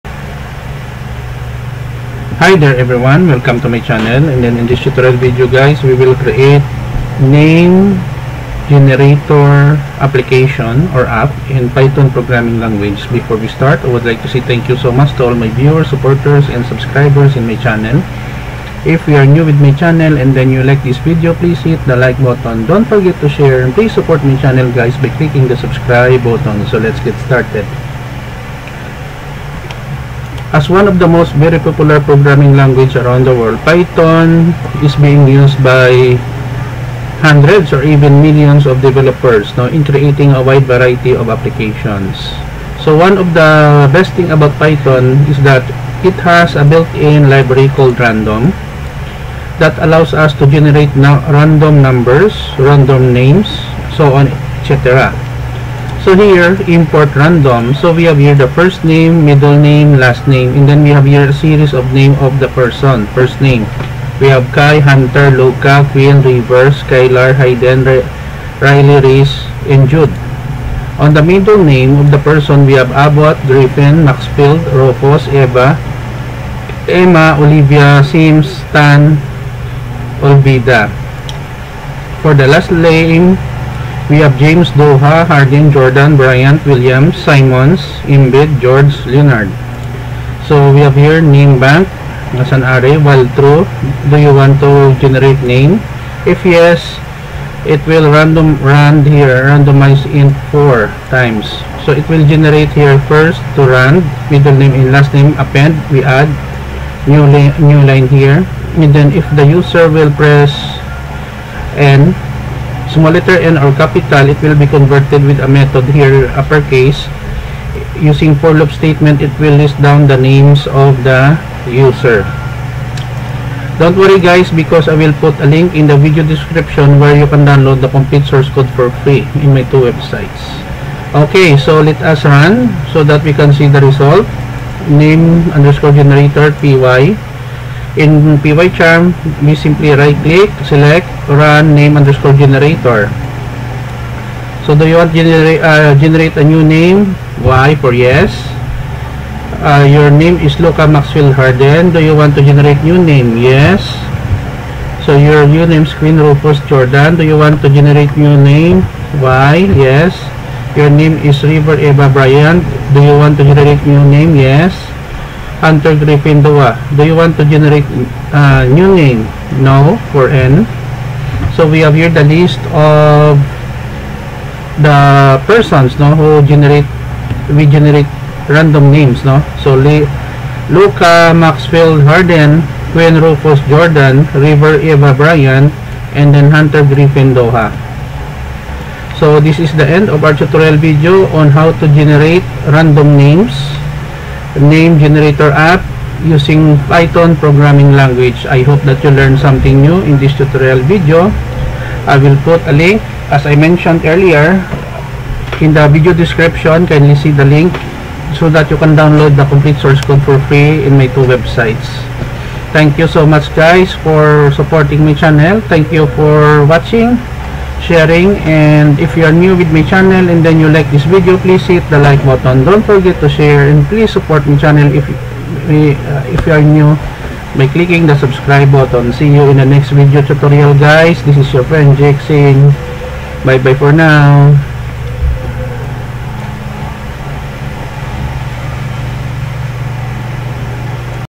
Name Generator App in Python
A simple program to generate random names using Python programming language.